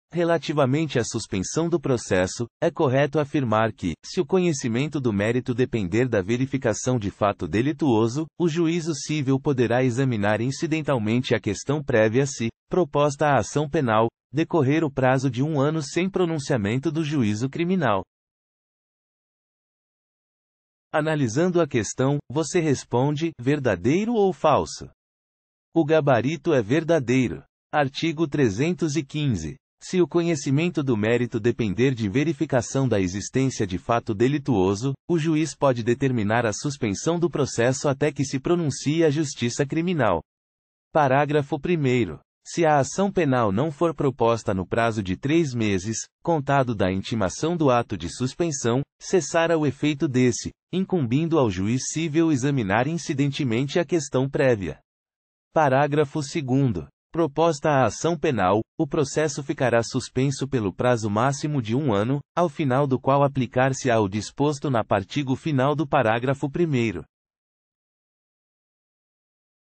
QScon Direito: [Art. 315, §§ 1° e 2°, CPC] TRF - 3ª Região 2018 - Juiz Federal
Art. 315, §§ 1° e 2°, CPC | TRF 3ª Região 2018 | Juiz Federal

Relativamente à suspensão do processo, é CORRETO afirmar que, se o conhecimento. do mérito depender da verificação de fato delituoso, o juízo cível poderá. examinar incidentalmente a questão prévia se, proposta a ação penal, decorrer o. prazo de um ano sem pronunciamento do juízo criminal.

Gabarito: Verdadeiro